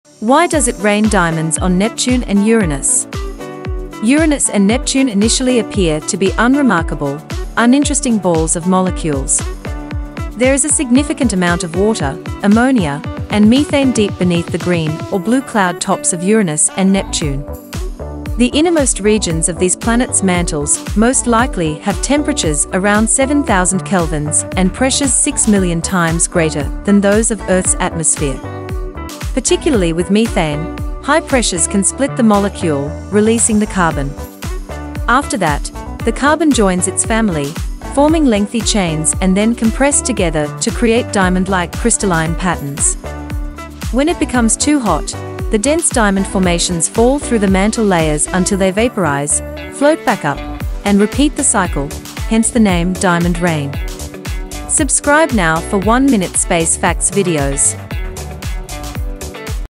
Why Does It Rain Diamonds On Neptune And Uranus ?
Uranus and Neptune initially appear to be unremarkable, uninteresting balls of molecules.
There is a significant amount of water, ammonia, and methane deep beneath the green or blue cloud tops of Uranus and Neptune.
The innermost regions of these planets' mantles most likely have temperatures around 7,000 kelvins and pressures 6 million times greater than those of Earth's atmosphere.
Particularly with methane, high pressures can split the molecule, releasing the carbon. After that, the carbon joins its family, forming lengthy chains and then compressed together to create diamond-like crystalline patterns.
When it becomes too hot, the dense diamond formations fall through the mantle layers until they vaporise, float back up, and repeat the cycle—hence the name "diamond rain."